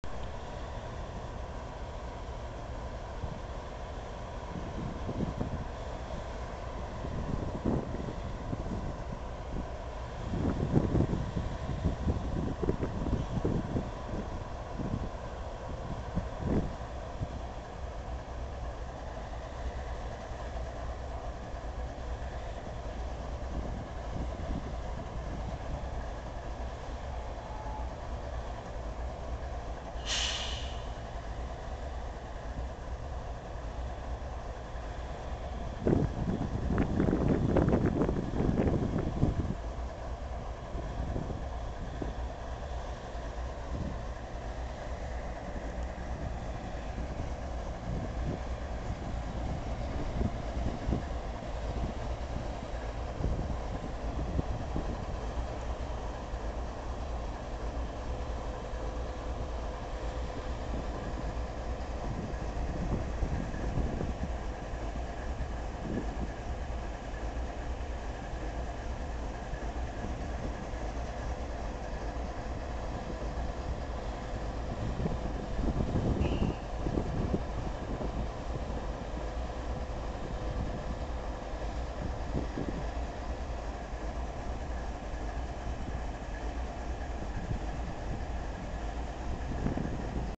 UP C45ACCTA #7373 idling
This big GE is on the back end of double stack train stop in sweetwater,tx.  I found it stop along a privat road that gose in the yard where i'm at in sweetwater,tx.